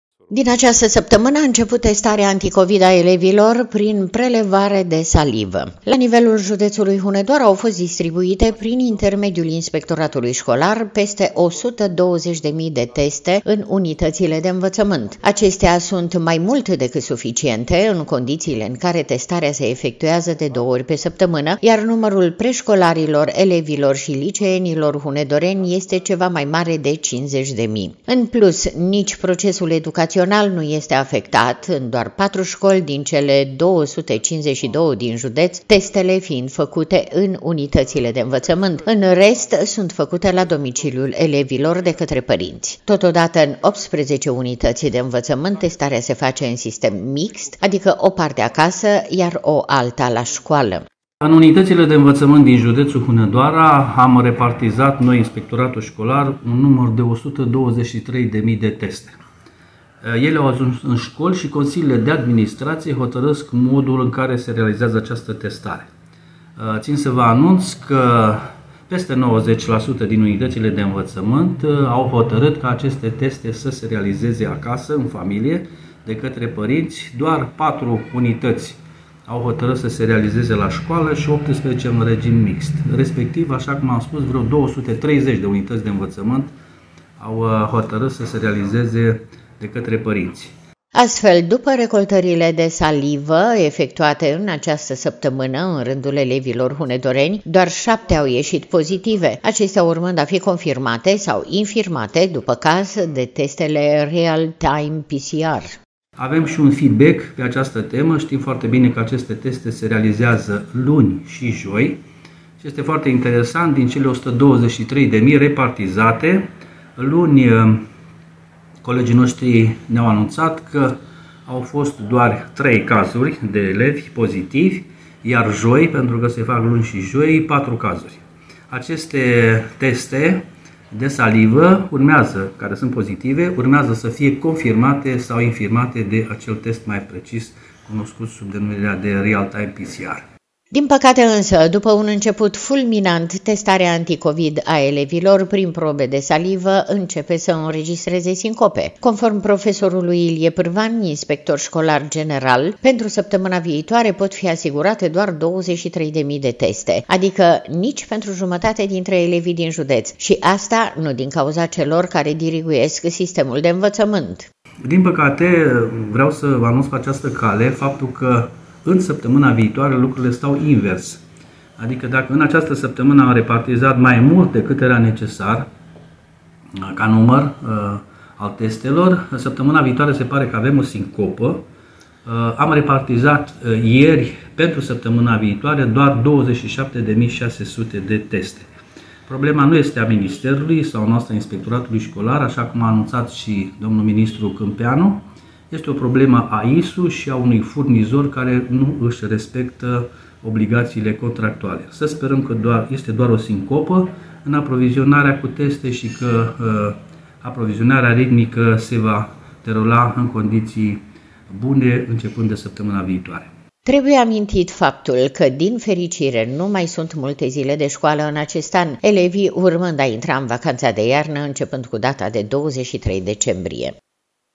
ÎNCEP SĂ APARĂ PROBLEME CU ASIGURAREA TESTELOR DE SALIVĂ PENTRU ELEVI
Din această săptămână a început testarea anticovid a elevilor prin prelevare de salivă. La nivelul județului Hunedoara au fost distribuite, prin intermediul Inspectoratului Școlar, peste 120.000 de teste în unitățile de învățământ. Acestea sunt mai mult decât suficiente, în condițiile în care testarea se efectuează de două ori pe săptămână, iar numărul preșcolarilor, elevilor și liceenilor hunedoreni este ceva mai mare de 50.000. În plus, nici procesul educațional nu este afectat, în doar 4 școli din cele 252 din județ testele fiind făcute în unitățile de învățământ, în rest fiind făcute la domiciliul elevilor, de către părinți. Totodată, în 18 unități de învățământ testarea se face în sistem mixt, adică o parte acasă, iar o alta la școală.